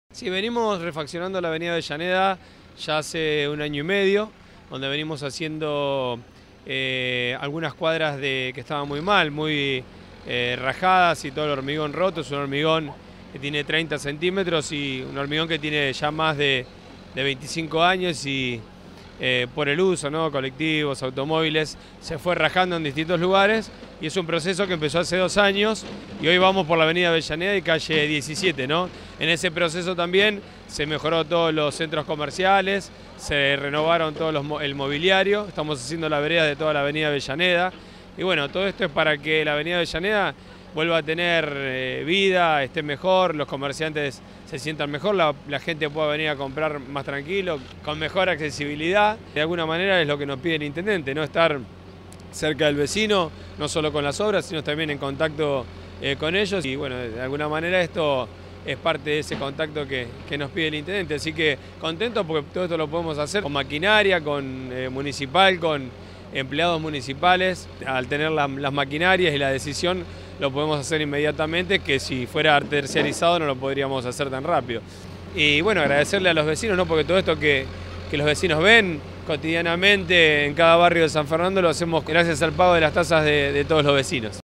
Renovamos el pavimento de Av. Avellaneda
El Presidente del HCD, Santiago Aparicio, supervisó las obras de refacción de la carpeta asfáltica y el hormigonado que tienen lugar a lo largo de la neurálgica vía de circulación que atraviesa el centro comercial de Virreyes. “En este proceso también se mejoró el centro comercial, con mobiliario y veredas nuevas. Todo para que vuelva a ser un lugar con mucha vida, que los comerciantes se sientan más a gusto y pase más gente”, afirmó.